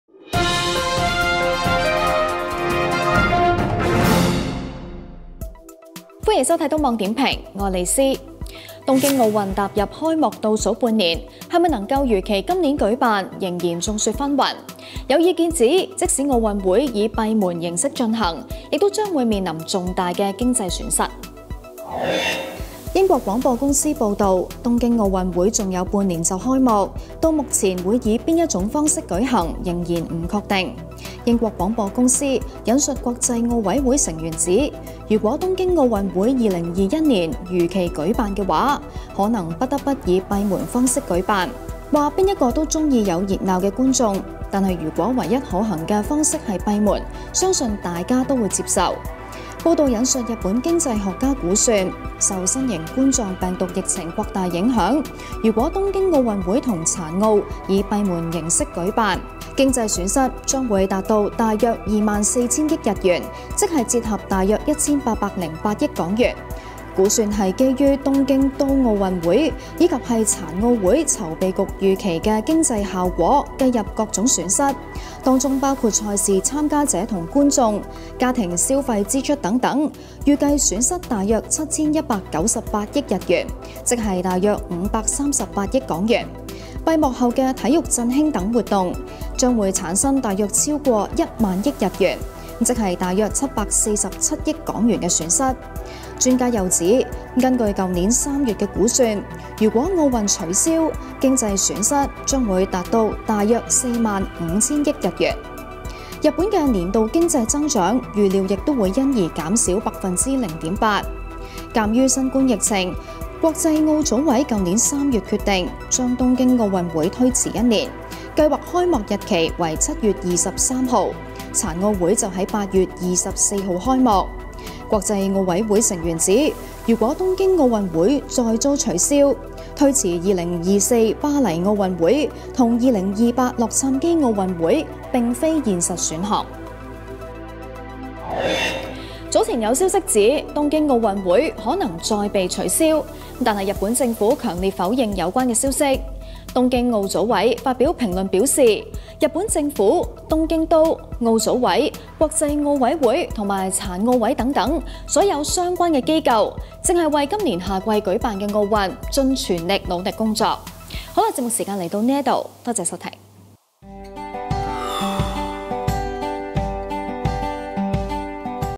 【on.cc東網】東網點評：東京奧運若閉門進行 料蝕近2000億港元
搞唔搞得成都成問題...

【東網點評：東京奧運若閉門進行　料蝕近2000億港元】